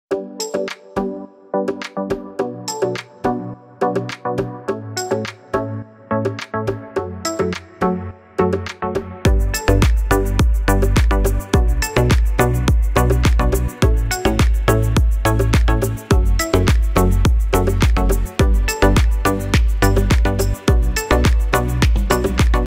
Doctor Given💉 injection in Hand Vein with care caught in Mobile - YouTube
In this video you will see how Doctor Given injection in Hand Vein with care caught in Mobile . You will vein injection is difficult from normal injection.